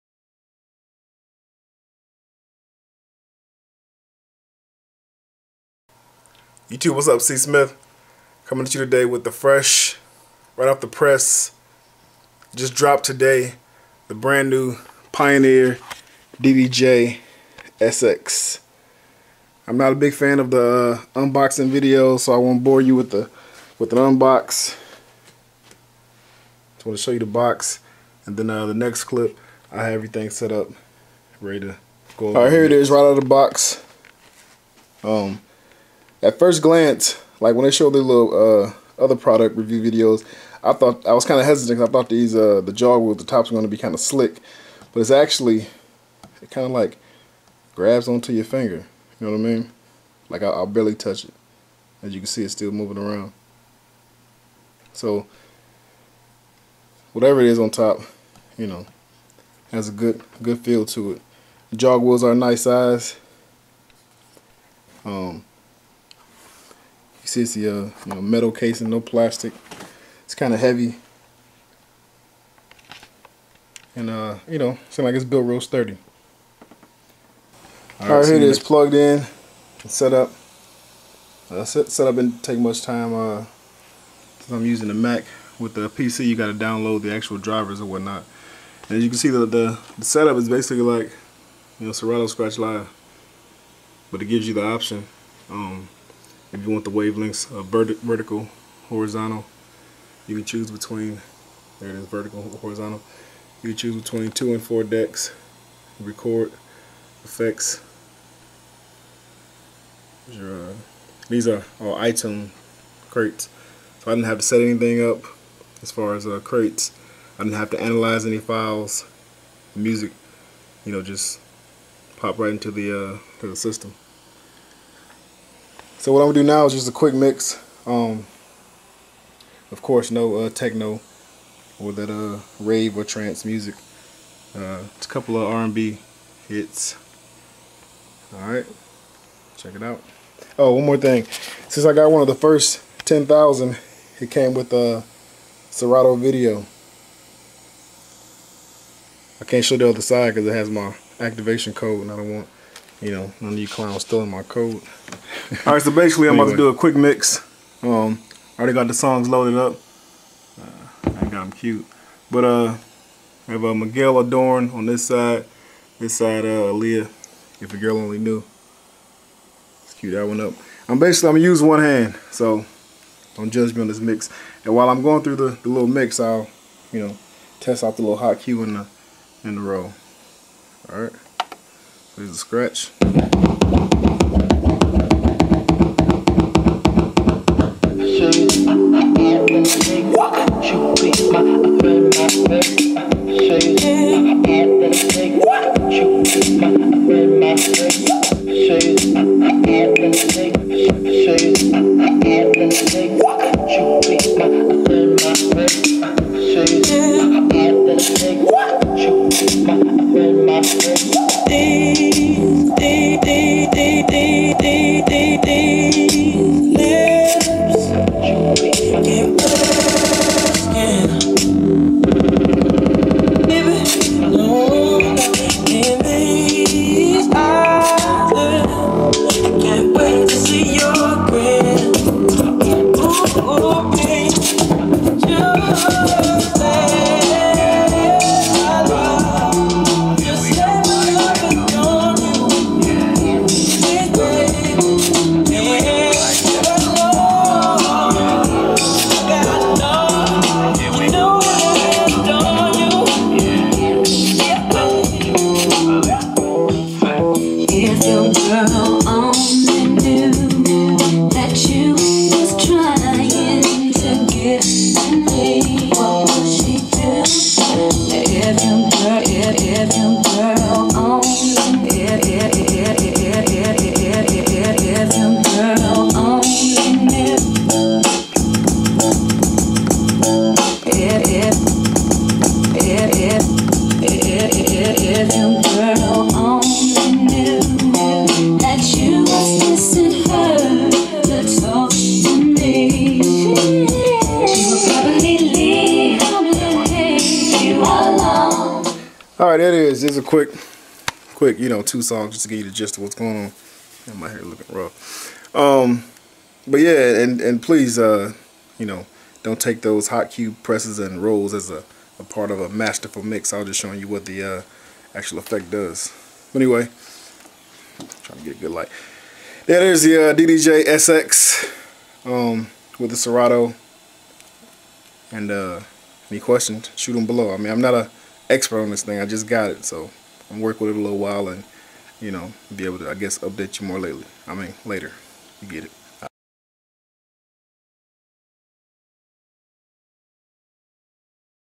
Pioneer DDJ-SX Mix & Review
A quick, and I do mean quick, look at the Pioneer DDJ-SX.